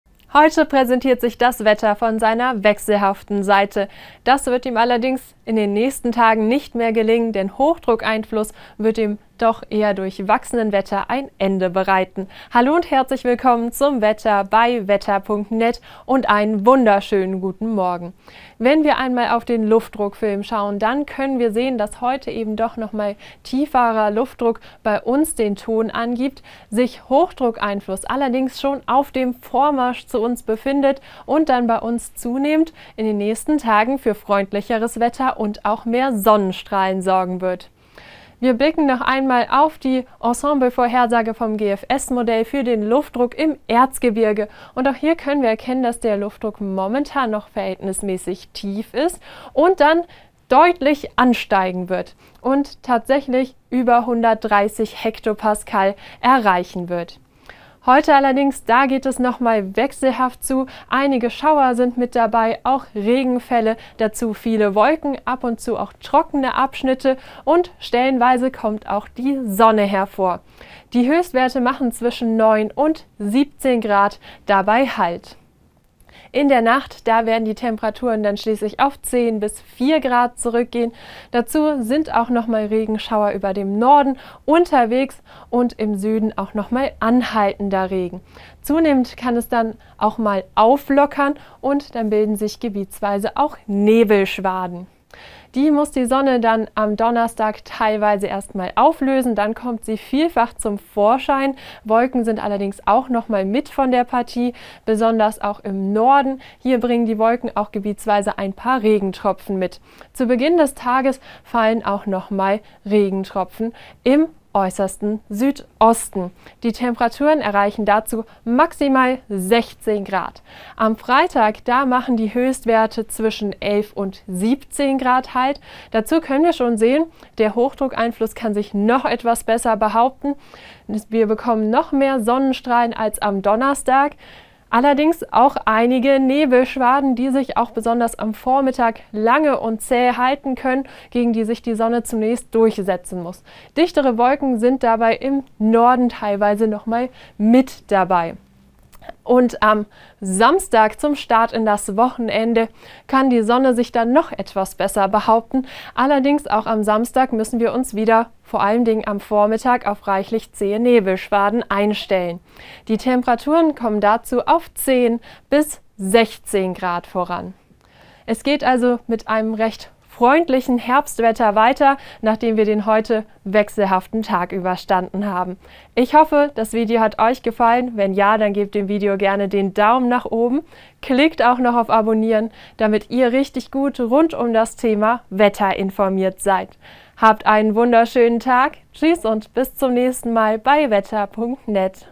Aufwärtstrend nach Tiefdruck: Neues Mega-Hoch im Anmarsch! Keine Chance fürs Winterwetter.
Das aktuelle Wetter für Deutschland vom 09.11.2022.

Heute setzt sich durchwachsenes Wetter durch. Das wird allerdings nicht von Dauer sein. Dank Hochdruckeinfluss wird es freundlicher. Mehr dazu erfahrt Ihr von Meteorologin B.Sc. Adrienne Jeske.